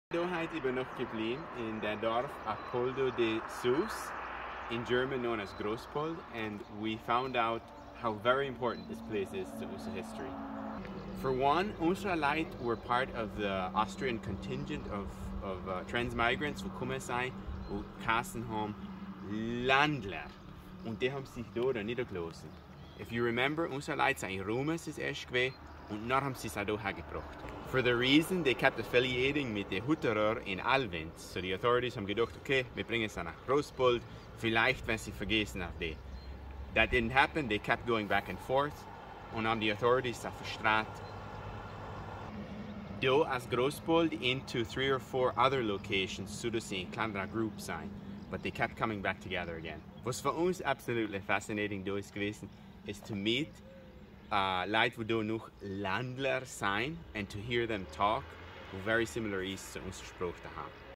Hutterite Pilgrimage: Grosspold (Alpoldo de Sus)
Jason talks about how important this place is to our history.